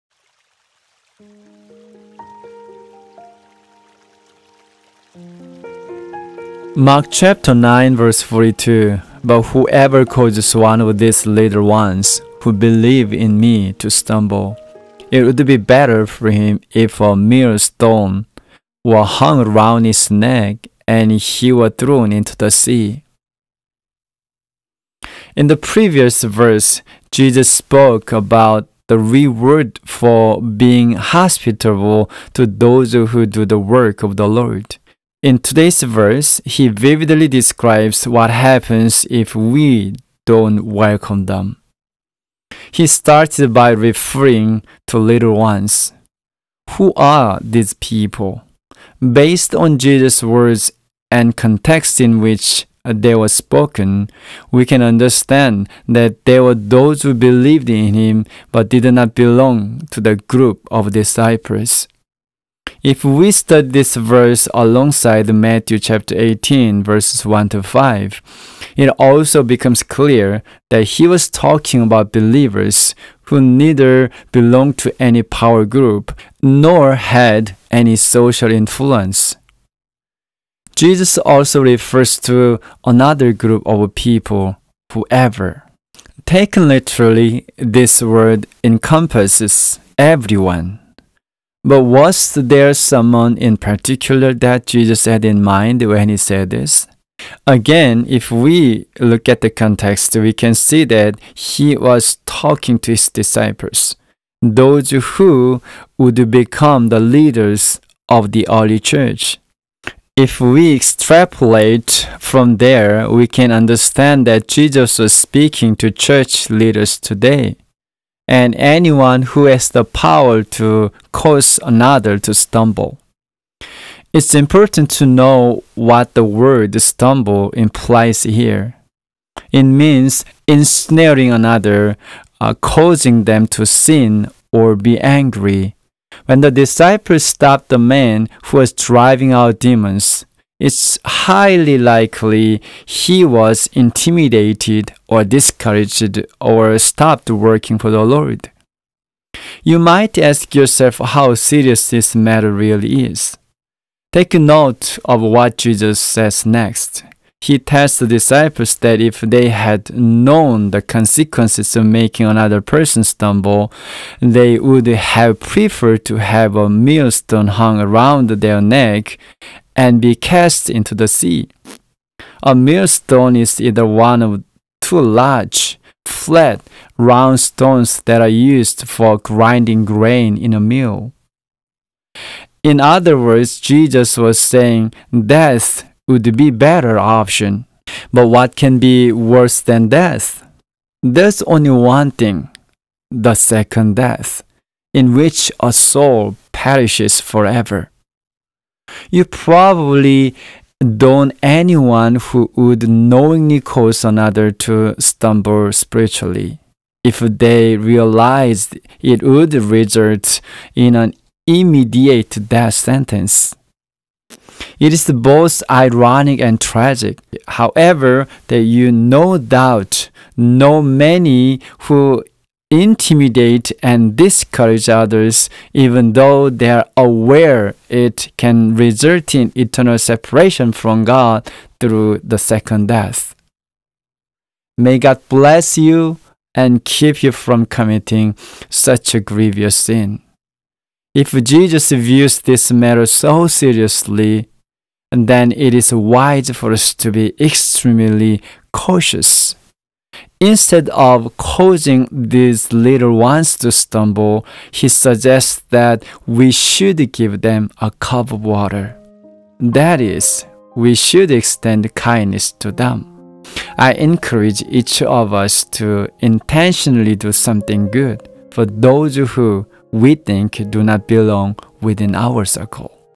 Morning Devotion- 104 Gospel of Mark- by Pastor Joel Kim
Mark 9:42
“But whoever causes one of these little ones who believe in Me to stumble, it would be better for him if a millstone were hung around his neck, and he were thrown into the sea.

In the previous verse, Jesus spoke about the reward for being hospitable to those who do the work of the Lord.  In today’s verse, he vividly describes what happens if we don’t welcome them.

He starts by referring to ‘little ones’.  Who are these people?  Based on Jesus’ words and the context in which they were spoken, we can understand that they were those who believed in Him but did not belong to the group of disciples.  If we study this verse alongside Matthew 18:1-5, it also becomes clear that He was talking about believers who neither belonged to any power group, nor had any social influence.

Jesus also refers to another group of people – ‘whoever’.  Taken literally, this word encompasses everyone, but was there someone in particular that Jesus had in mind when He said this?  Again, if we look at the context, we can see that he was talking to His disciples, those who would become the leaders of the early church.  If we extrapolate from there, we can understand that Jesus was speaking to church leaders today, and anyone who has the power to cause another to stumble.

It’s important to know what the word ‘stumble’ implies here.  It means ensnaring another, causing them to sin or be angry.  When the disciples stopped the man who was driving out demons, it’s highly likely he was intimidated or discouraged, and stopped working for the Lord.

You might ask yourself how serious this matter really is.  Take note of what Jesus says next.  He tells the disciples that if they had known the consequences of making another person stumble, they would have preferred to have a millstone hung around their neck, and be cast into the sea.  A millstone is “either one of two large, flat, round stones that are used for grinding grain in a mill.”  (Merriam Webster Dictionary).  In other words, Jesus was saying death would be a better option!  But what can be worse than death?  There is only one thing – the second death, in which a soul perishes forever.

You probably don’t anyone who would knowingly cause another to stumble spiritually if they realised it would result in an immediate death sentence.  It is both ironic and tragic, however, that you no doubt know many who intimidate and discourage others even though they are aware it can result in eternal separation from God through the second death.

May God bless you and keep you from committing such a grievous sin!

If Jesus views this matter so seriously, then it is wise for us to be extremely cautious.  Instead of causing these little ones to stumble, He suggested that we should give them a cup of water.  That is, we should extend kindness to them.

I encourage each of us to intentionally do something good for those who we think do not belong within our circle.